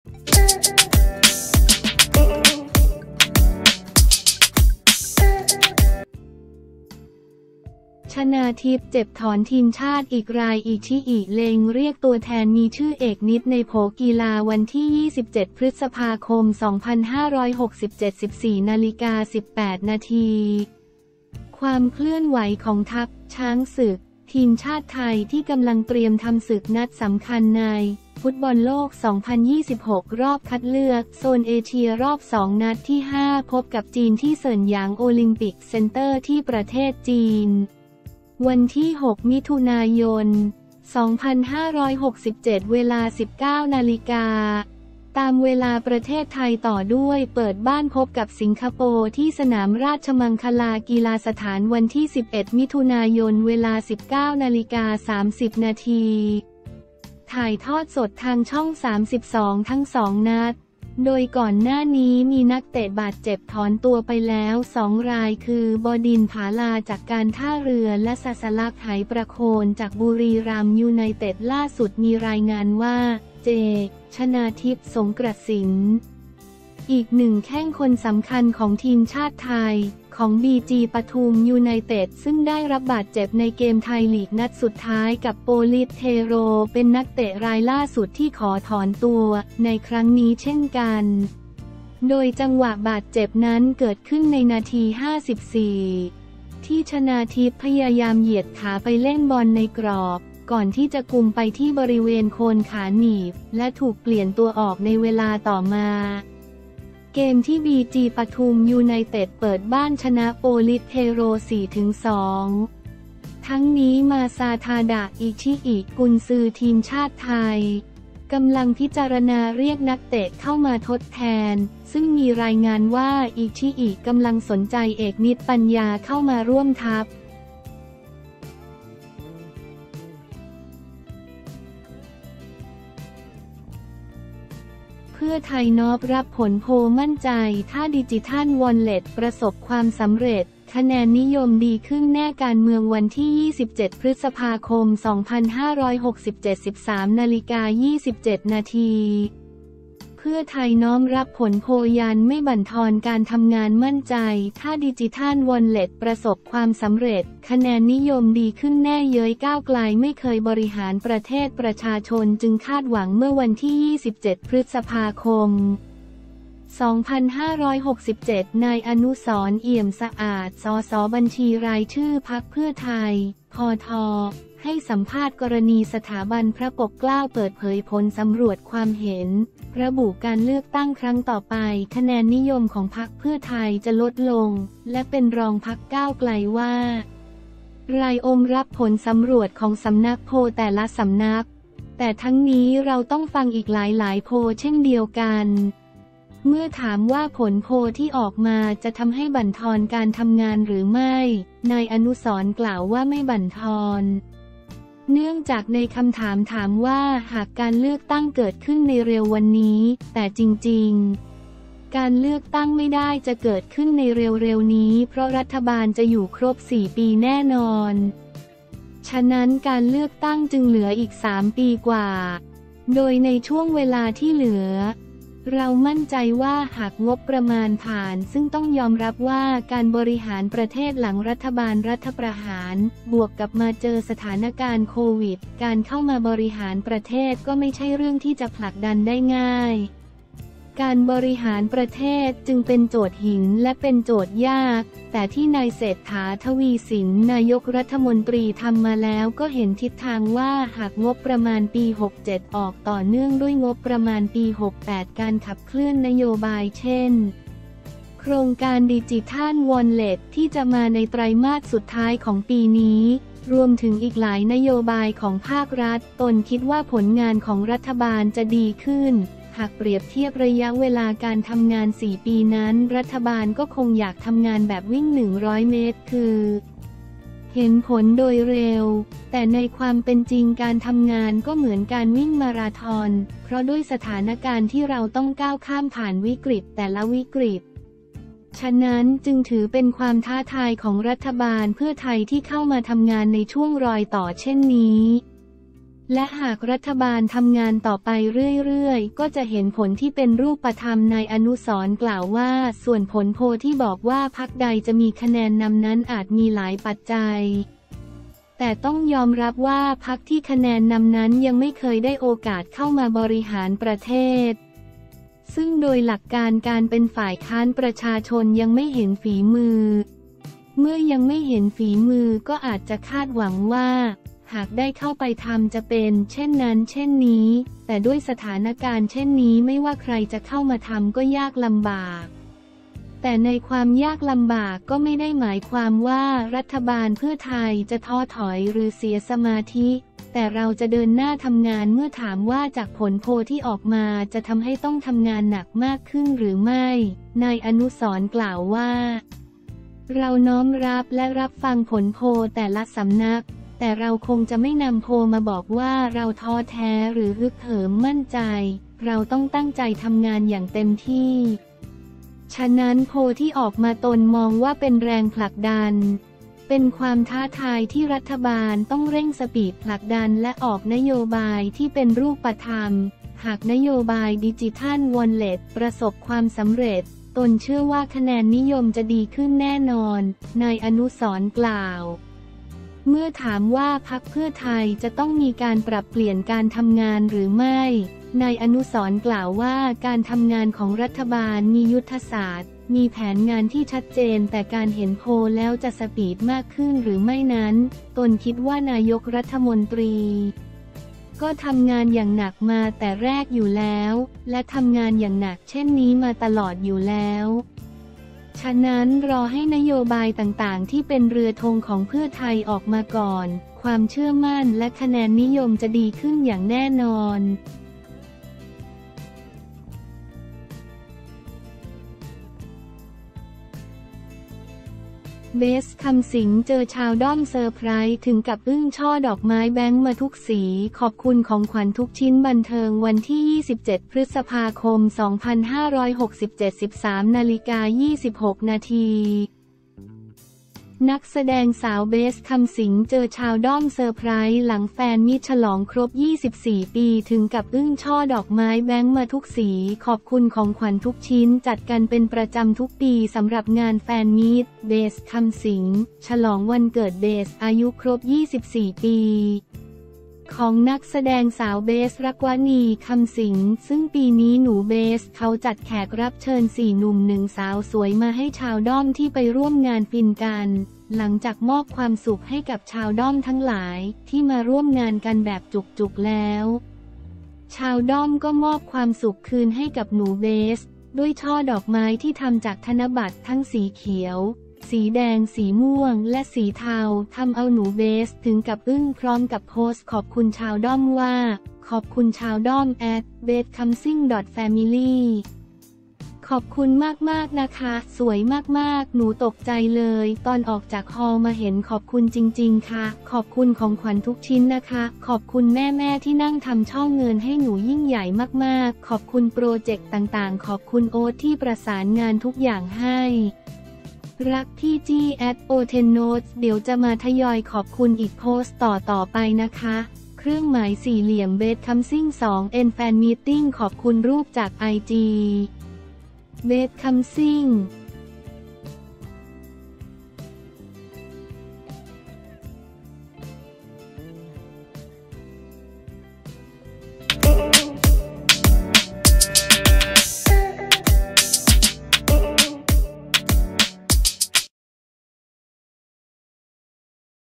ชนาธิป เจ็บถอนทีมชาติอีกราย อิชิอิ เล็งเรียกตัวแทน มีชื่อ เอกนิษฐ์ ในโผ
กีฬา
27 พ.ค. 2567
14:18 น.
ความเคลื่อนไหวของทัพ “ช้างศึก” ทีมชาติไทย ที่กำลังเตรียมทำศึกนัดสำคัญใน ฟุตบอลโลก 2026 รอบคัดเลือก โซนเอเชีย รอบ 2 นัดที่ 5 พบกับ จีน ที่ เสิ่นหยาง โอลิมปิก เซ็นเตอร์ ที่ ประเทศจีน วันที่ 6 มิถุนายน 2567 เวลา 19.00 น. ตามเวลาประเทศไทย ต่อด้วยเปิดบ้านพบกับสิงคโปร์ ที่ สนามราชมังคลากีฬาสถาน วันที่ 11 มิถุนายน เวลา 19.30 น. ถ่ายทอดสดทางช่อง 32 ทั้งสองนัด

โดยก่อนหน้านี้ มีนักเตะบาดเจ็บ ถอนตัวไปแล้ว 2 รายคือ บดินทร์ ผาลา จากการท่าเรือ และศศลักษณ์ ไหประโคน จากบุรีรัมย์ ยูไนเต็ด
ล่าสุดมีรายงานว่า “เจ” ชนาธิป สรงกระสินธ์ อีกหนึ่งแข้งคนสำคัญของทีมชาติไทย ของ บีจี ปทุม ยูไนเต็ด ซึ่งได้รับบาดเจ็บในเกมไทยลีก นัดสุดท้าย กับ โปลิศ เทโร เป็นนักเตะรายล่าสุดที่ขอถอนตัว ในครั้งนี้เช่นกัน

โดยจังหวะบาดเจ็บนั้น เกิดขึ้นในนาที 54 ที่ ชนาธิป พยายามเหยียดขาไปเล่นบอลในกรอบ ก่อนที่จะกุมไปที่บริเวณโคนขาหนีบ และถูกเปลี่ยนตัวออกในเวลาต่อมา เกมที่ บีจี ปทุม ยูไนเต็ด เปิดบ้านชนะ โปลิศ เทโร 4-2

ทั้งนี้ มาซาทาดะ อิชิอิ กุนซือทีมชาติไทย กำลังพิจารณาเรียกนักเตะเข้ามาทดแทน ซึ่งมีรายงานว่า อิชิอิ กำลังสนใจ เอกนิษฐ์ ปัญญา เข้ามาร่วมทัพ